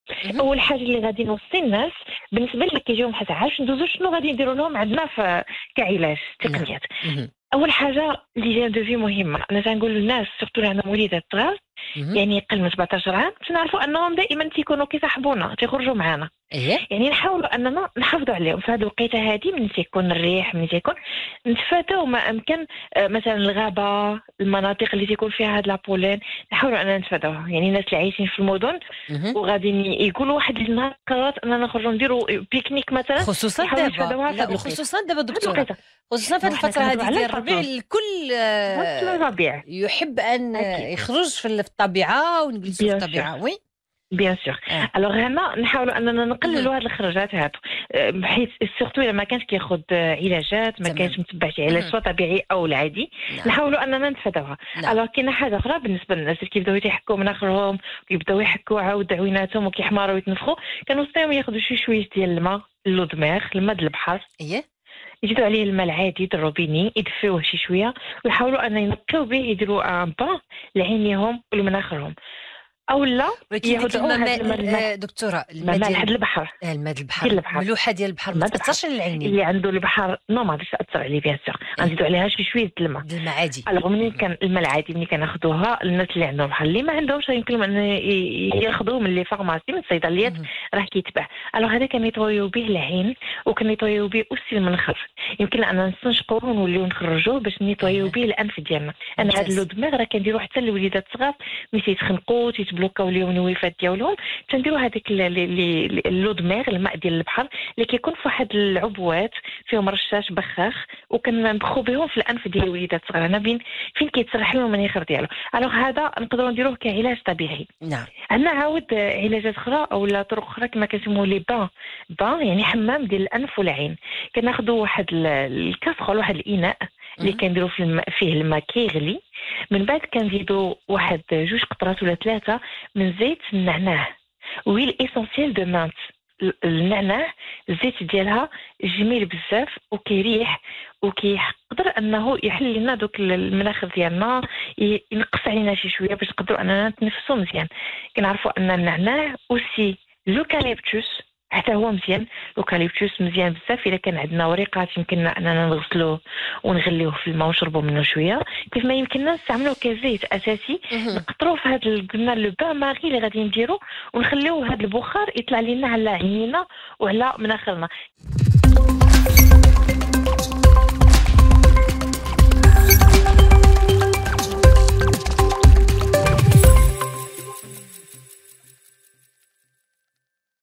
حساسية الربيع ..علاجات طبيعية
Vérifié
كيف تتم معالجة حساسية فصل الربيع بطرق طبيعية، للتخلص من حكة_ الأنف و